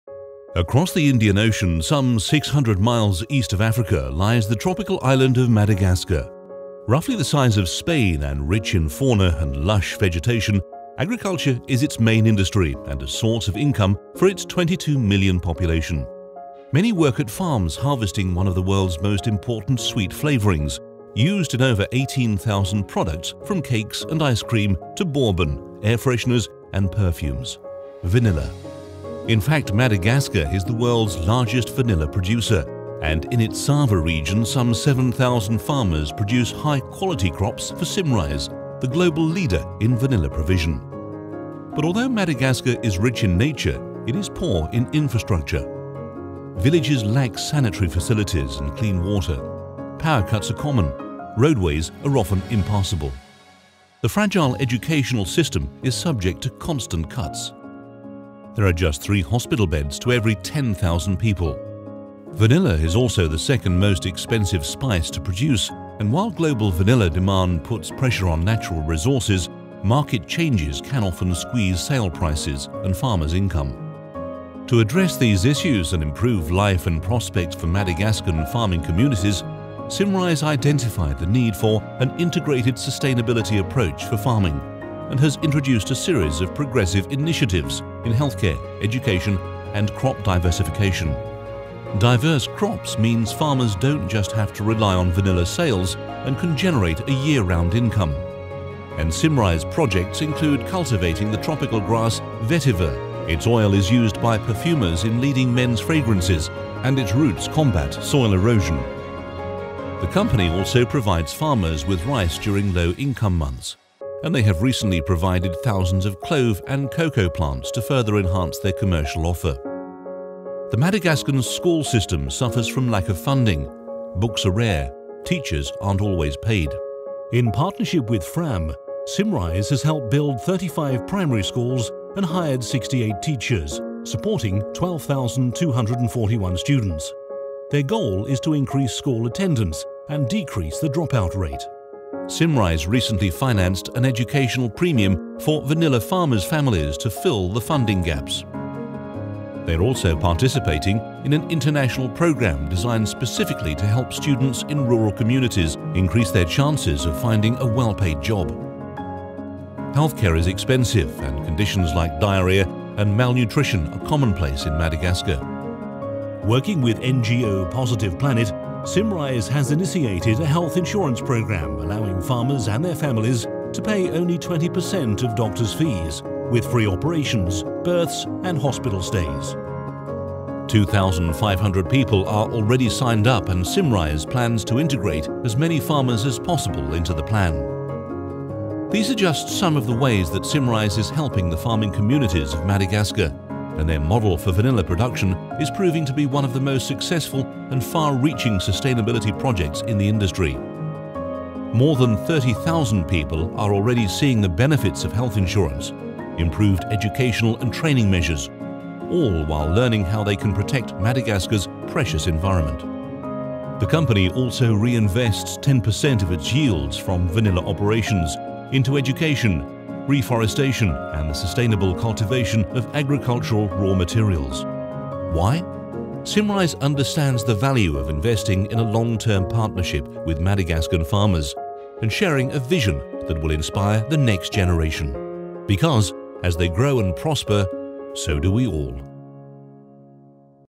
Madagascar - our sustainable engagement (long version)
For approx. 22 million people on Madagacar agriculture is the main industry and the source of income. Many work at farms harvesting one of the world's most important sweet flavorings: vanilla. 7,000 farmers produce high quality crops for Symrise. To improve life and prospects for Madagascan farming communities, Symrise identified the need for an integrated sustainability approach for farming, and has introduced a series of progressive initiatives in healthcare, education and crop diversification.